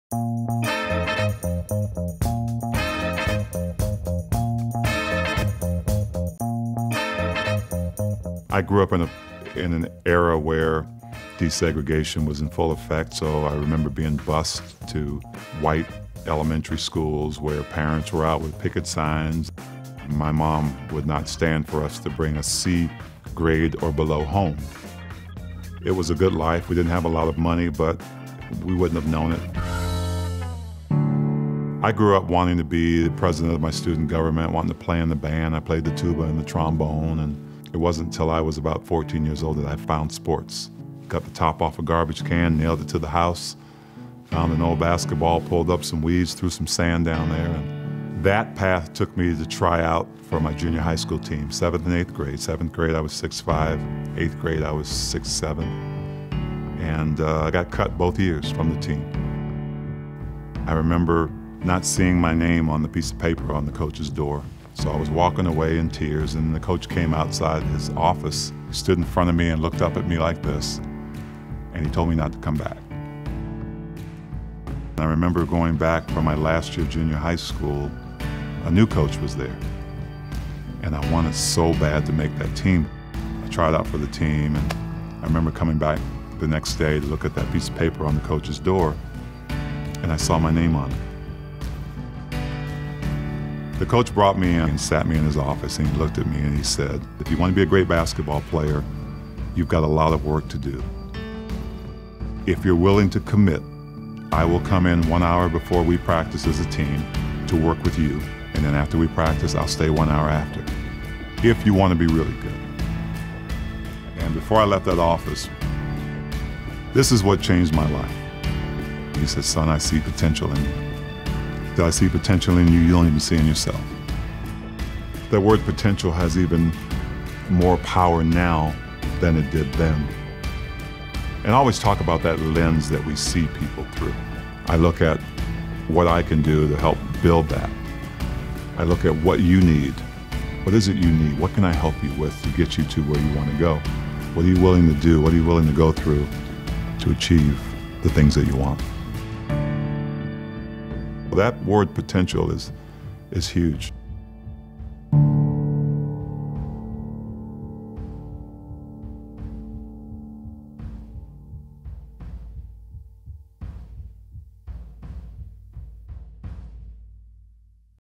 Thurl Bailey: "Power in Potential"
As a 6'7" eighth-grader, Thurl Bailey was cut from his school's basketball team and the coach told him to never come back. The next year, a new coach offered him a spot on the team because of the potential he saw in Thurl. Within eight years, Thurl Bailey had won an NCAA Championship at North Carolina State and was drafted by the NBA's Utah Jazz--in large part because one coach saw in him what others couldn't.